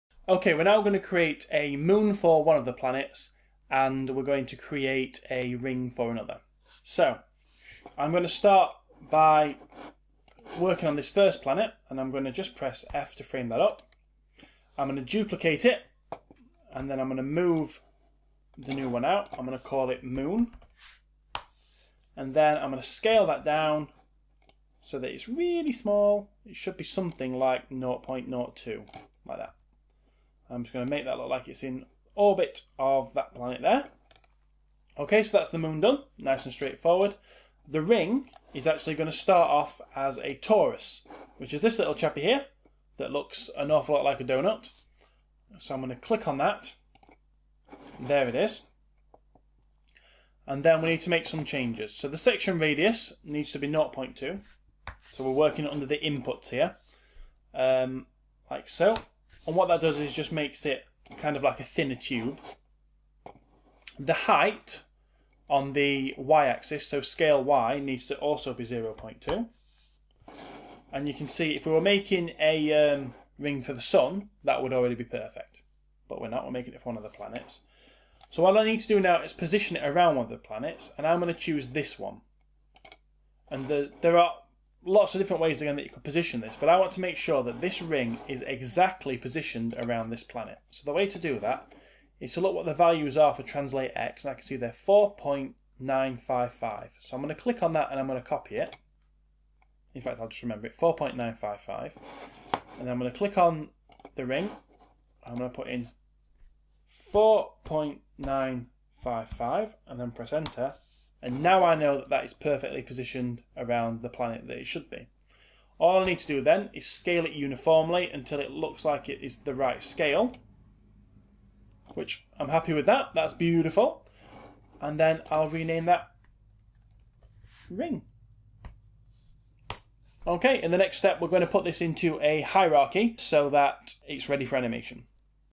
#21 Adding a moon and a ring
In the part of my Maya 2016 beginners tutorial you will add a ring to one of the planets using a torus and will duplicate one of the planets to make a moon.